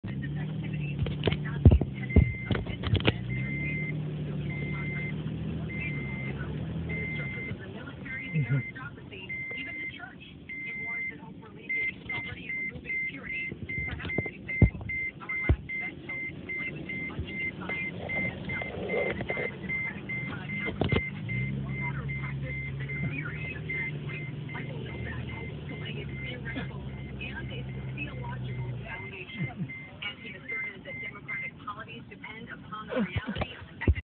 A 500-pound great white shark was caught and released from Sunset Beach. The video shows the shark as it gets back into the ocean. Video courtesy of Huntington Beach Marine Safety Department.